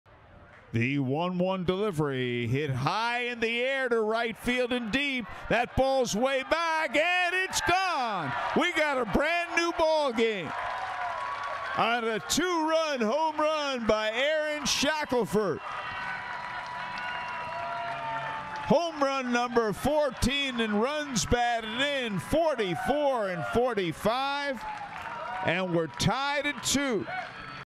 Shackelford Two-Run Blast - 7/28/23
Aaron Shackelford launched a two-run home run on Friday night at Victory Field.